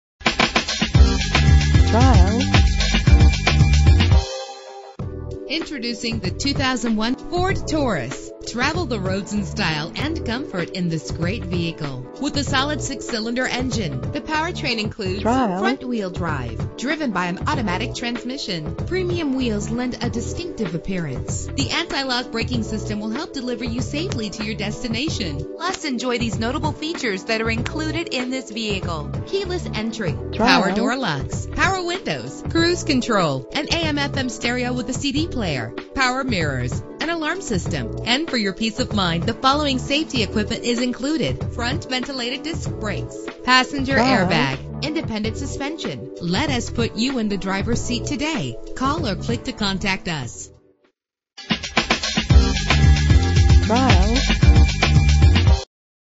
2001 Ford Taurus - Belmont NH
Year: 2001
Make: Ford
Model: Taurus
Trim: SES
Engine: 3.0 liter 6 cylinder 24 valve
Transmission: 4-Speed Automatic
Color: Tan
Mileage: 116717
Address:
29 Laconia Road, Route 106